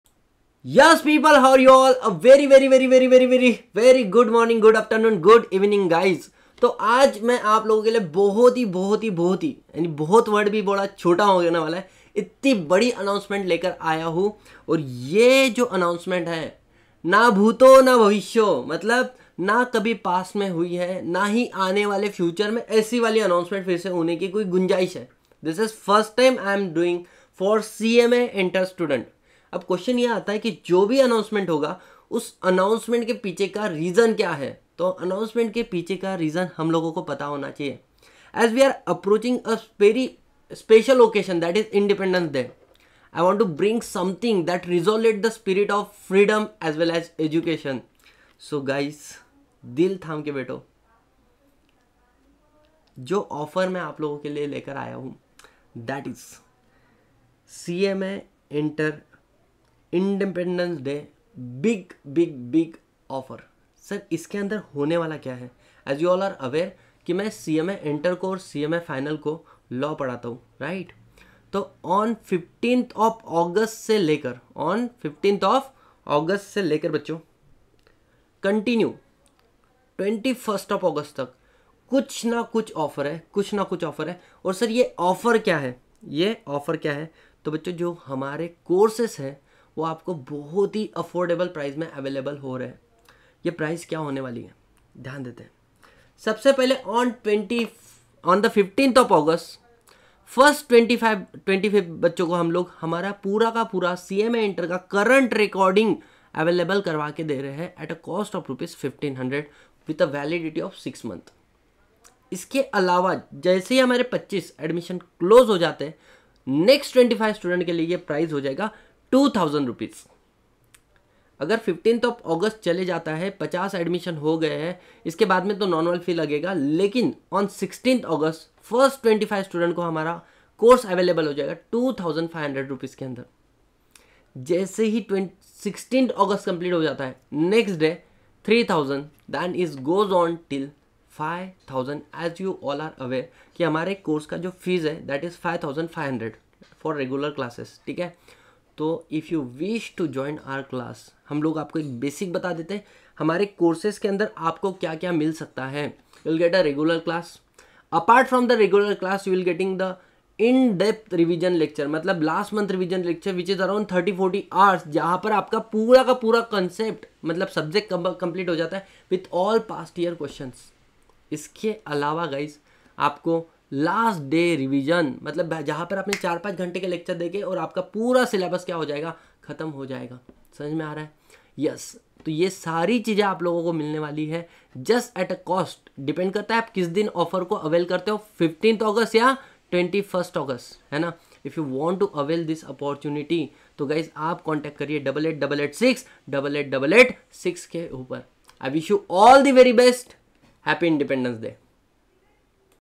CMA Inter | Independence Day Offer | By CS Satish Baheti | Agrawal Classes
Hello Students,
CS Satish Sir is announcing a big offer combo for CMA Inter Law. Grab it now!

Agrawal Classes, Pune's Premium CA, CS, and CMA Classes. Face-to-Face Batches, Google Drive and Pendrive Recorded Video Lectures, and Live Classes are all available.